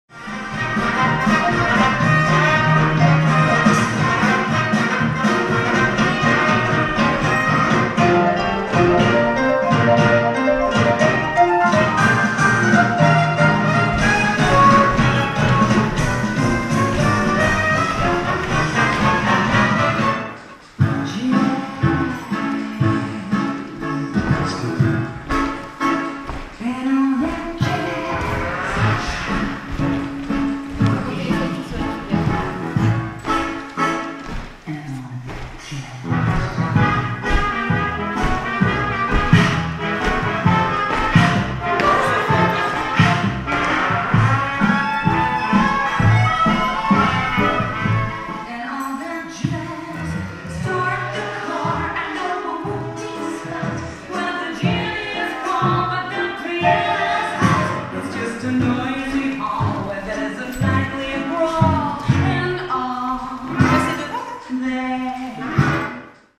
Modern' jazz (répétition)
Foyer rural des jeunes de Mondragon (Vaucluse) -
Section Modern' jazz animée par Noëlle Garcia - Répétition - Samedi 28 novembre 2009